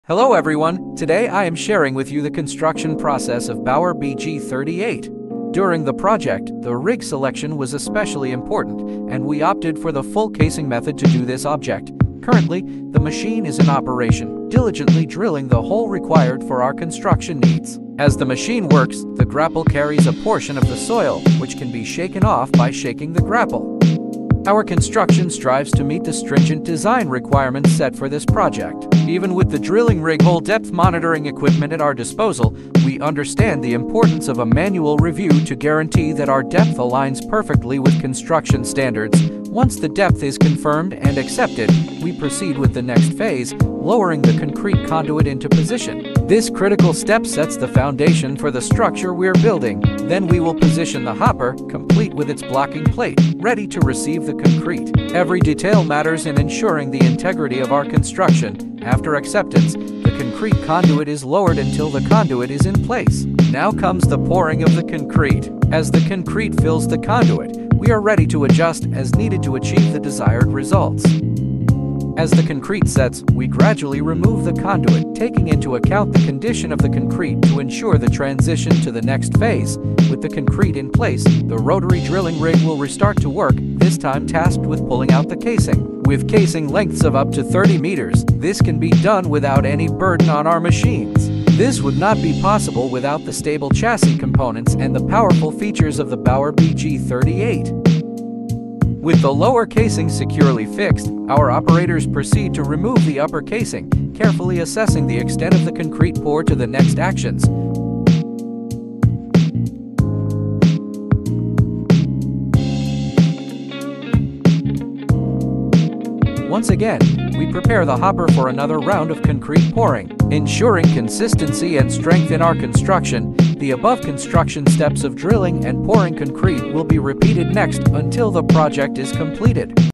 The construction process of Bauer BG38 Drilling rig machine
Hello everyone, today I am sharing with you the construction process of Bauer BG38.  During the project, the rig selection was especially important, and we opted for the full casing method to do this object.

Currently, the machine is in operation, diligently drilling the hole required for our construction needs. As the machine works, The grapple carries a portion of the soil, which can be shaken off by shaking the grapple.

Our construction strives to meet the stringent design requirements set for this project. Even with the drilling rig hole depth monitoring equipment at our disposal, we understand the importance of a manual review to guarantee that our depth aligns perfectly with construction standards.

Once the depth is confirmed and accepted, we proceed with the next phase: lowering the concrete conduit into position. This critical step sets the foundation for the structure we're building.

Then we will position the hopper, complete with its blocking plate, ready to receive the concrete. Every detail matters in ensuring the integrity of our construction.

After acceptance, the concrete conduit is lowered until the conduit is in place. Now comes the pouring of the concrete. As the concrete fills the conduit, we are ready to adjust as needed to achieve the desired results.

As the concrete sets, we gradually remove the conduit, taking into account the condition of the concrete to ensure the transition to the next phase.

With the concrete in place, the rotary drilling rig will restart to work,  this time tasked with pulling out the casing. With casing lengths of up to 30 meters, This can be done without any burden on our machines.

With the lower casing securely fixed, our operators proceed to remove the upper casing, carefully assessing the extent of the concrete pour to the next actions.

Once again, we prepare the hopper for another round of concrete pouring, ensuring consistency and strength in our construction.

The above construction steps of drilling and pouring concrete will be repeated next until the project is completed.